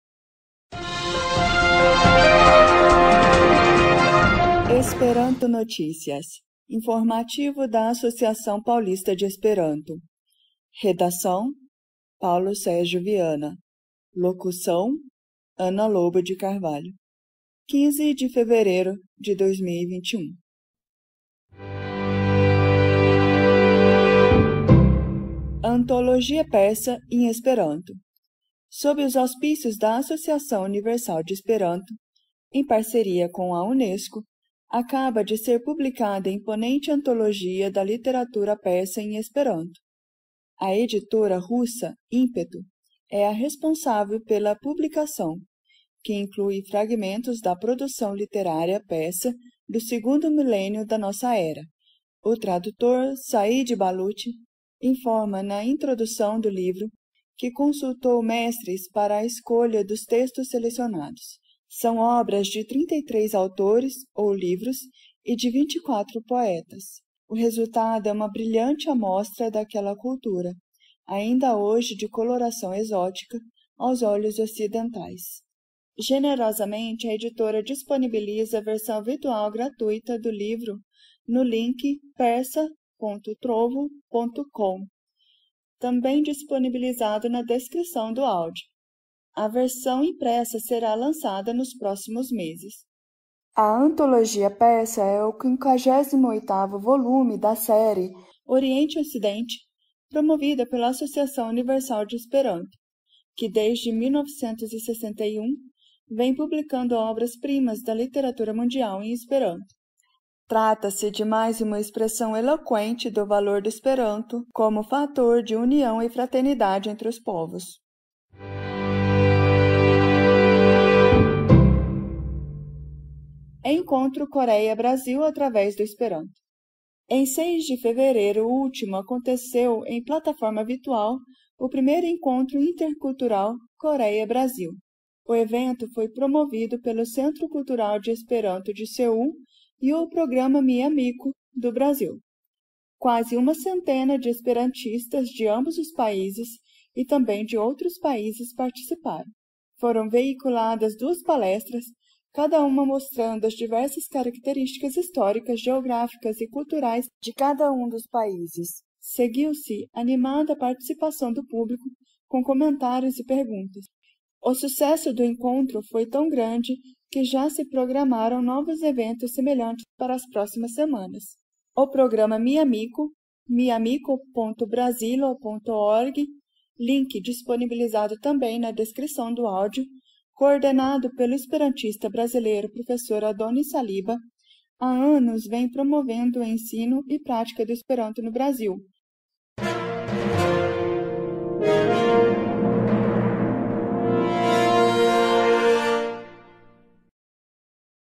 EASP Esperanto Notícias 15.02.2021
Redação: Paulo Sérgio Viana
Edição de som e Locução: Anna Lobo de Carvalho
Design Gráfico de Imagem: José Roberto Tenório
Edição de Vídeo: James Piton

Neste episódio
* ANTOLOGIA PERSA EM ESPERANTO
Link para download gratuita do livro: persa.trovu.com

* ENCONTRO COREIA-BRASIL ATRAVÉS DO ESPERANTO
link para o programa de ensino do esperanto no brasil que coordena o projeto: miaamiko.brazilo.org

Contato
easp.org.net